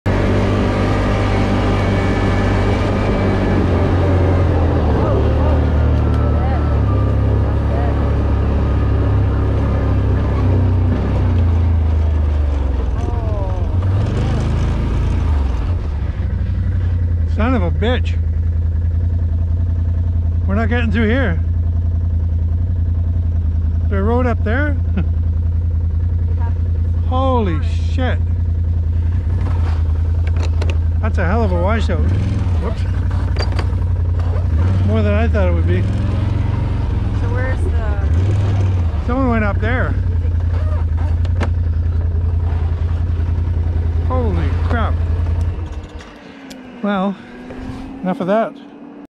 Virgin Falls Slide
No access to Virgin Falls near Pacific Rim Park, BC.